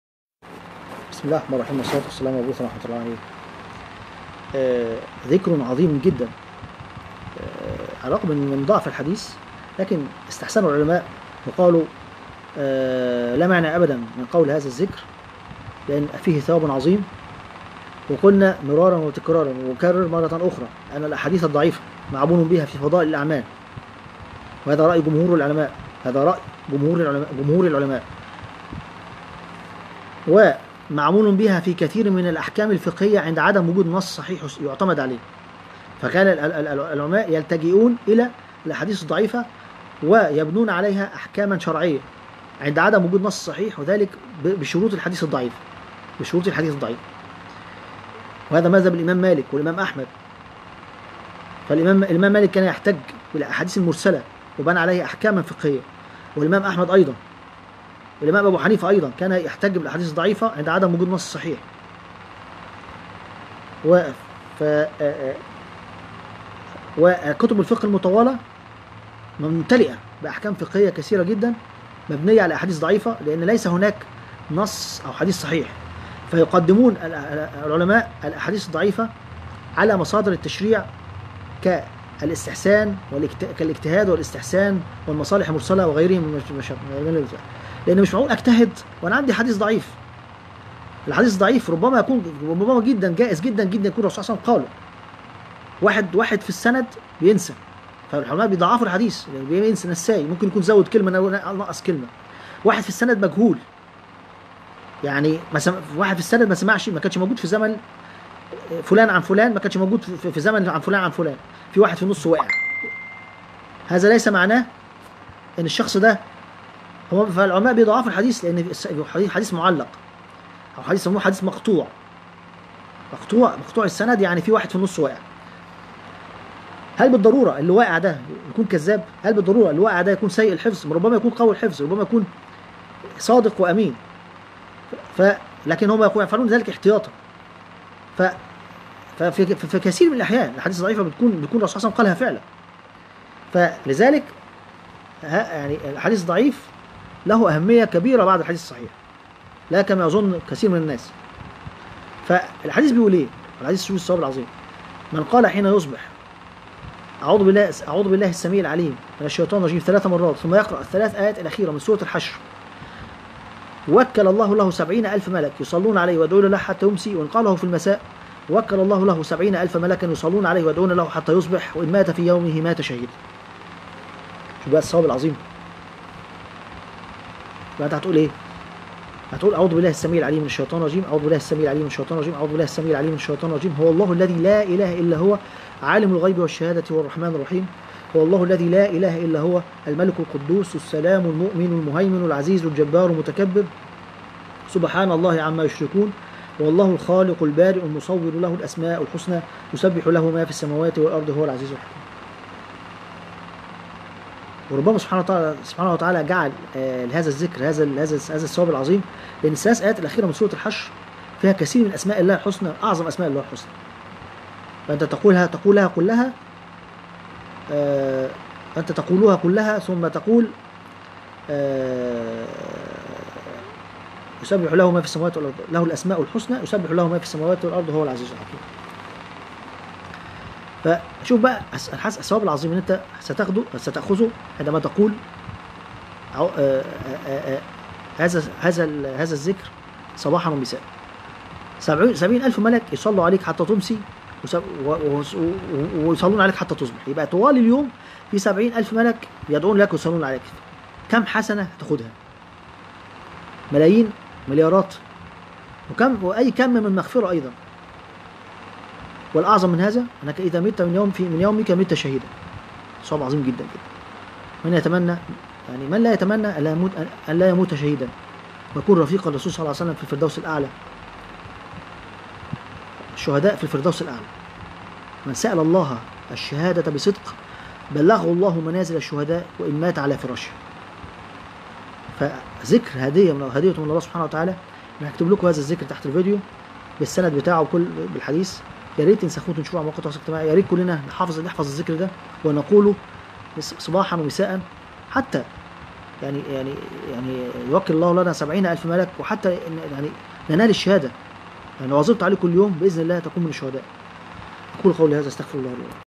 ذكر تنال به الشهادة و يصلي عليك به 70000 ملك طوال اليوم قراءة وصف الفيديو هام جداً جداً جداً جداً
قناة الشيخ حاتم مسعد
ذكر عظيم جداً جداً جداً يرجي نشره
روي الترمذي وأحمد والدارمي عن معقل بن يسار عن النبي صلى الله عليه وسلم قال: "من قال حين يصبح ثلاث مرات: أعوذ بالله السميع العليم من الشيطان الرجيم، وقرأ ثلاث آيات من آخر سورة الحشر، وكل الله به سبعين ألف ملك يصلون عليه حتى يمسي، وإن مات في ذلك اليوم مات شهيداً، ومن قالها حين يمسي كان بتلك المنزلة